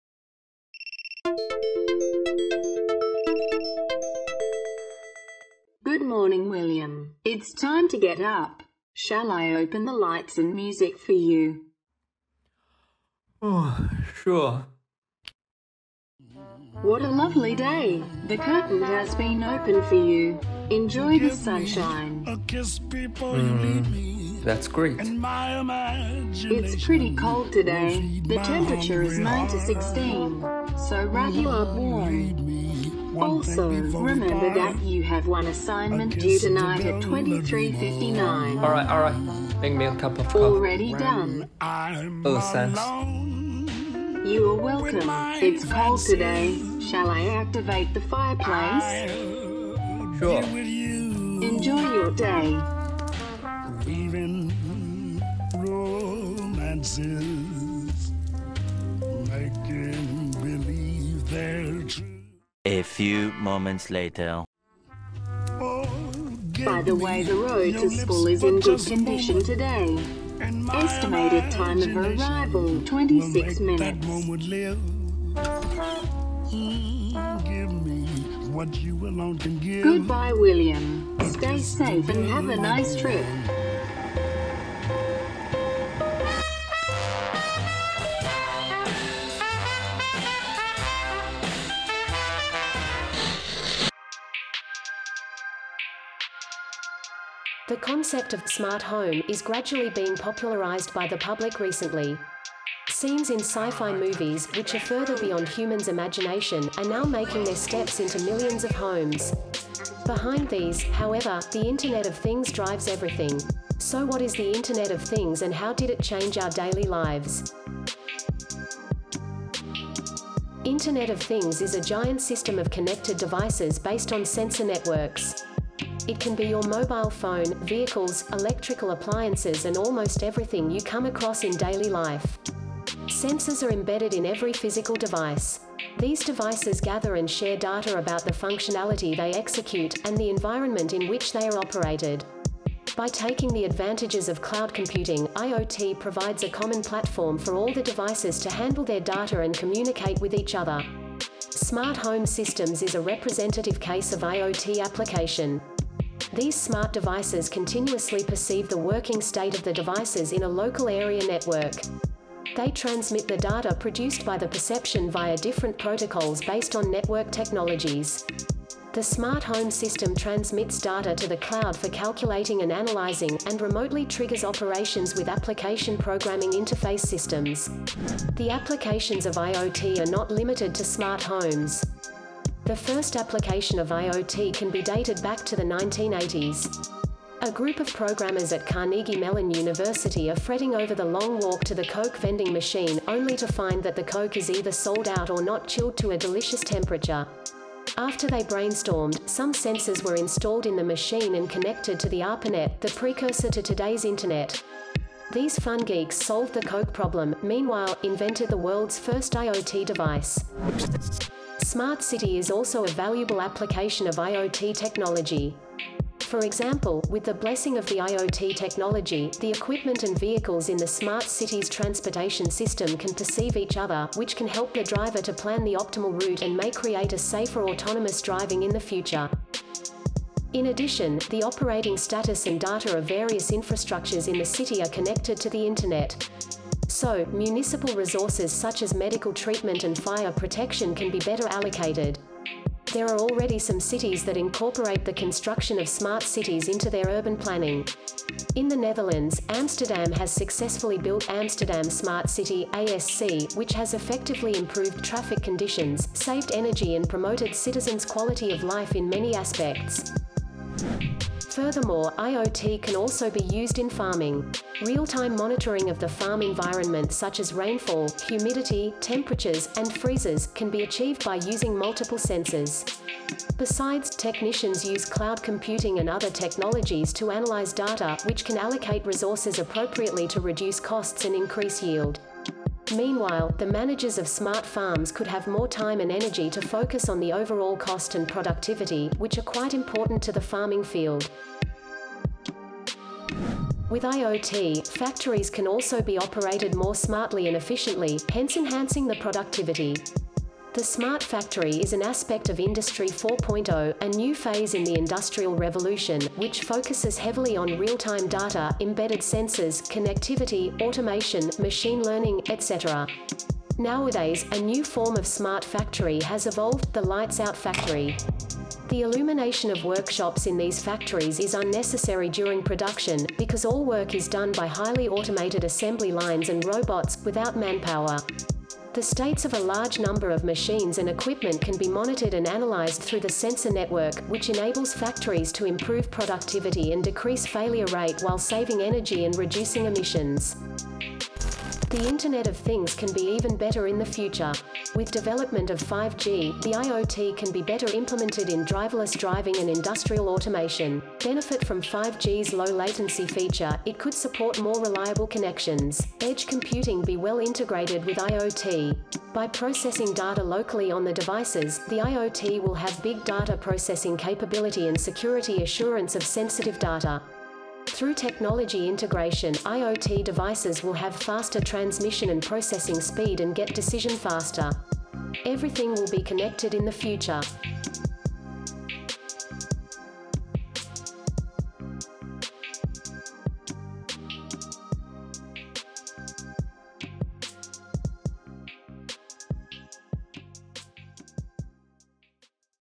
IoT: The Magic Behind The Smart Home
Group 170 Video
Binbin Tang, Jia Zhu, Quan Zhou, Weilun Chen, Xinnan Shen, and Zongdong Liu
School of Computing and Information Systems, University of Melbourne

{binbint, jiaz8, quanzhou, weilun, xinnan.shen,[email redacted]